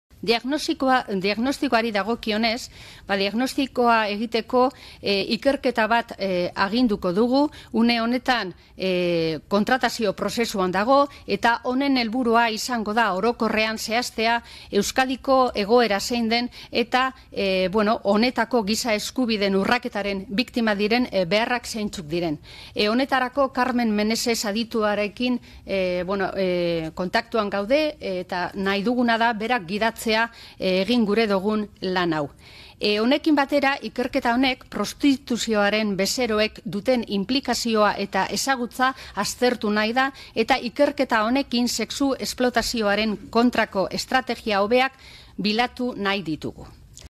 Gizakien salerosketari aurre egiteko 2018ko lan plana
Elkarrekin Podemos-eko legebiltzarkidea den Eukene Arana Varas-en galdera Emakundeko zuzendaria den Izaskun Landaida Larizgoitiari Eusko Legebiltzarreko Berdintasun eta Giza Eskubideen batzordean 2018ko martxoaren 7an. Galdera eta erantzun osoa (19 min).

- Elkarrekin Podemos-eko legebiltzarkidea den Eukene Arana Varas-en galderaren ondorengo galdera txanda (37 min)

Sexu-esplotazioa helburu duen gizakien salerosketaren biktima direnentzat urgentziazko etxebizitza bat martxan jarriko dela iragarri du Emakundek

Emakundeko zuzendariak, Izaskun Landaidak, agerraldia egin du gaur Eusko Legebiltzarreko Giza Eskubide eta Berdintasun Batzordean eta bertan azaldu ditu pertsonen sexu salerosketaren kontra egiteko egiteko sortutako mahaian hartutako zenbait erabaki. Mahai honetan parte hartzen dute erakunde publikoak zein gizarte eragileak. Emakundek koordinatzen duen mahaian, Ertzaintzako zuzendaritzak, Enplegu eta Gizarte Politiketarako sailak, Espainiako Gobernuaren Ordezkaritzak, Fiskaliak eta foru aldundiek parte hartu dute; eta hauekin batera, baita ere hainbat gizarte eragilek: Cáritas, CEAR, Munduko Medikuak, Ihesaren aurkako batzordea, Gizarterako, Askabide, Oblatas eta Accem.
Landaidak iragarri du egoeraren diagnostiko bat egingo dela, eta honekin batera, urte bukaerarako martxan jarriko dela urgentziazko harrera etxebizitza bat. Etxebizitza honek titularitate publikoa izango du baina hirugarren sektoreko elkarteek gestionatuko dute. Landaidak esker bereziak eman nahi izan dizkie alor honetan lan egiten ari diren hirugarren sektoreko elkarteei.